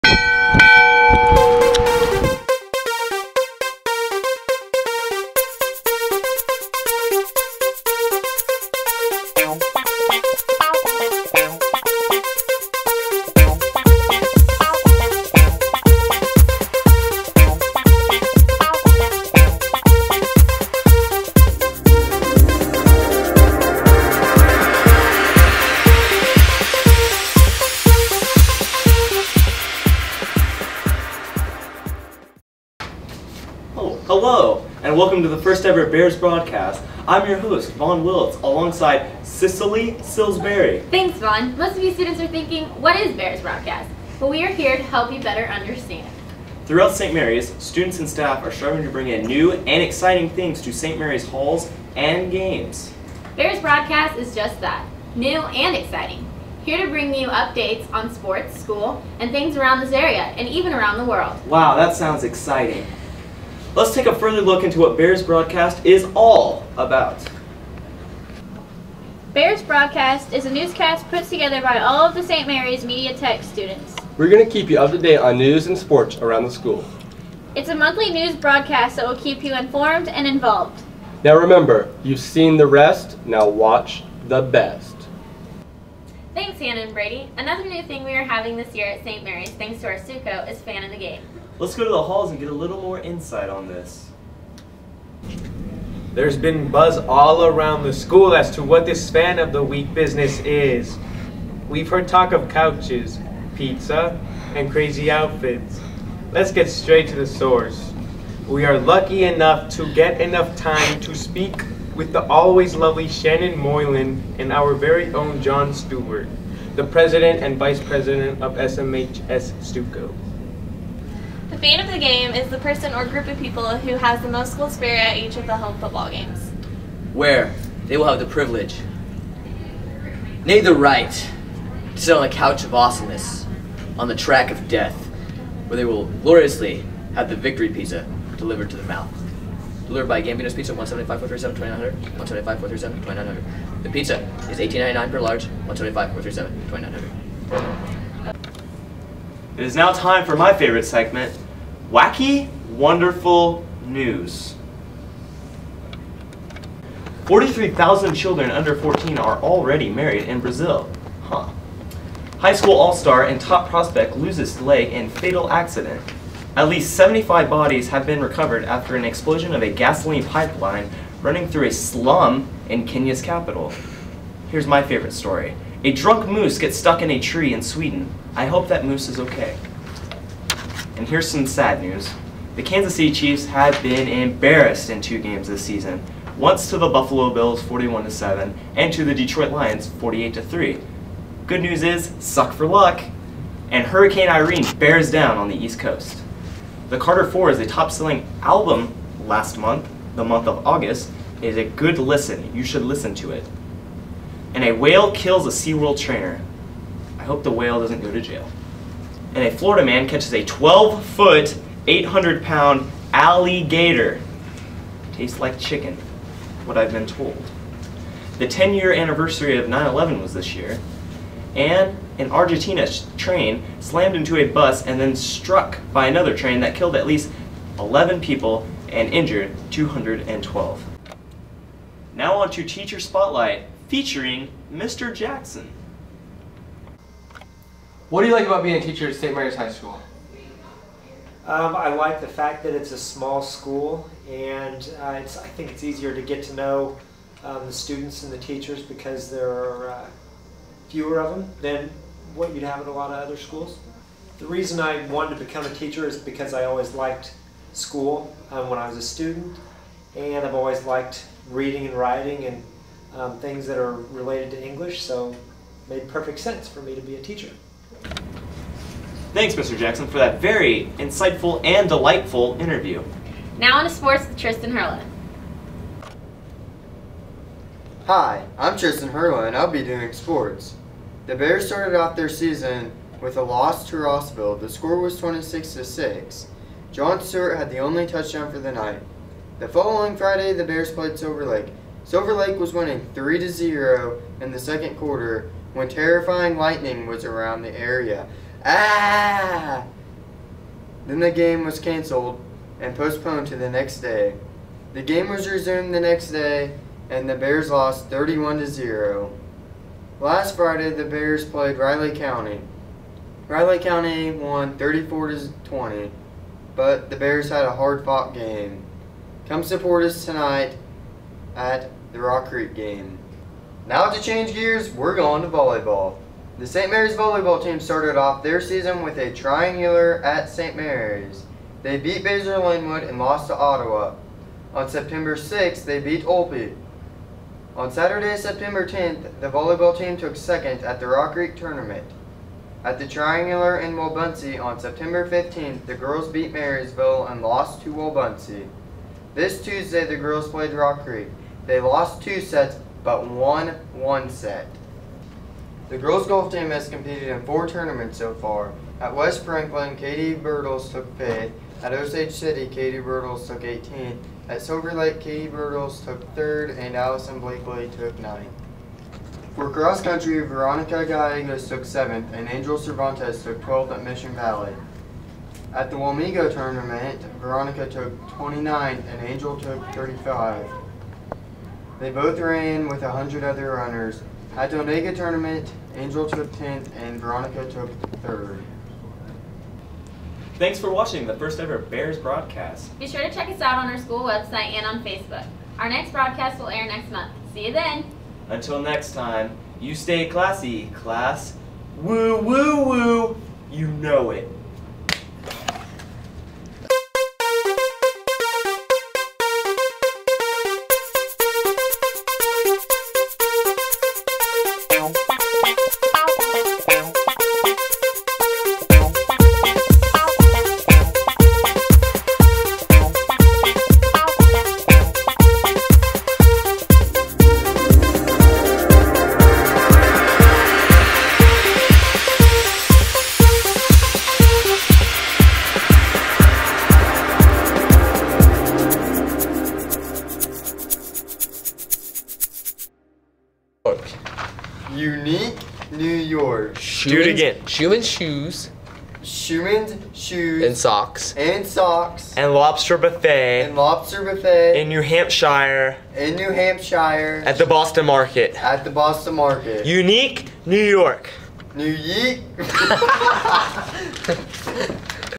Bears Broadcast Sept 2011
First ever Bears Broadcast, a monthly news broadcast created by the St. Marys High School Media Technology class.